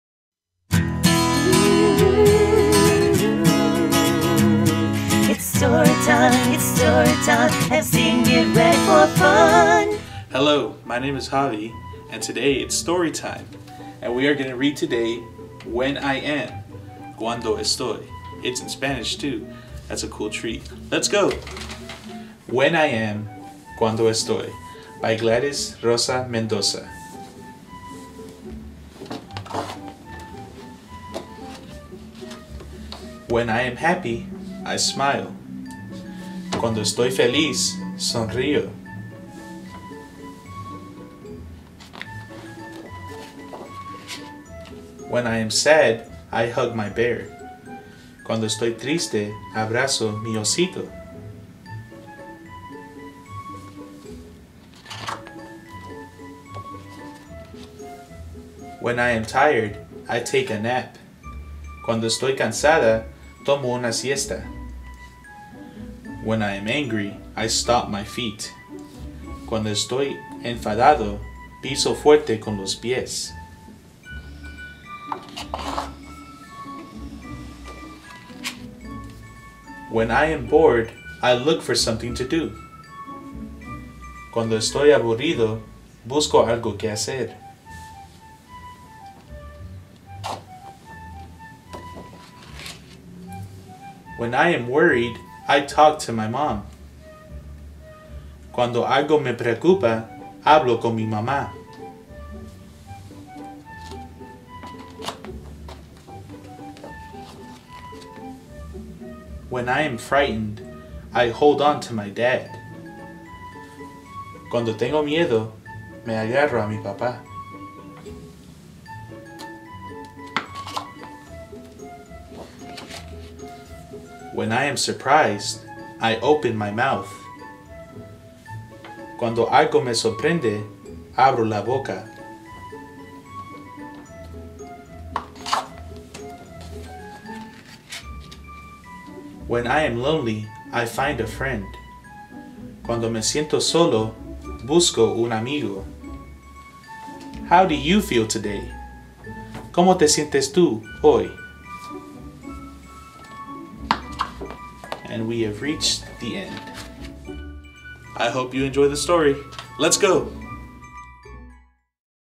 Storytime! Book: "When I Am/Cuando Estoy"
Georgetown County Library has a story for you! Today, Javier reads "When I Am/Cuando Estoy" by Gladys Rosa-Mendoza. It is a BIG book with lots of fun pictures!